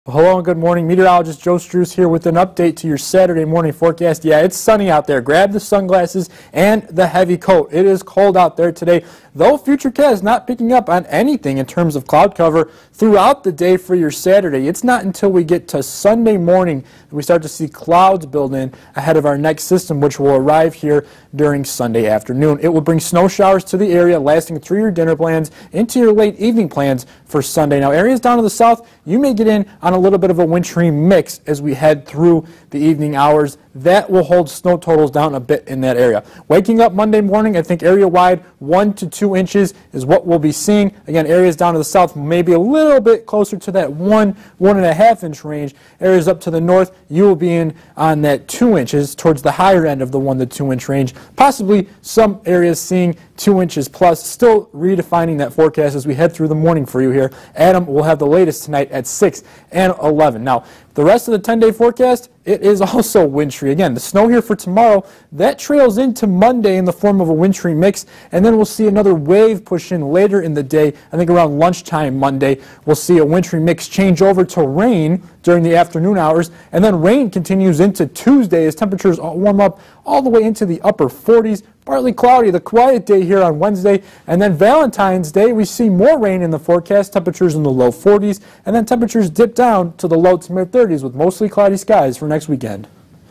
Saturday Morning Forecast Update
Sun continues to shine today, snow arrives Sunday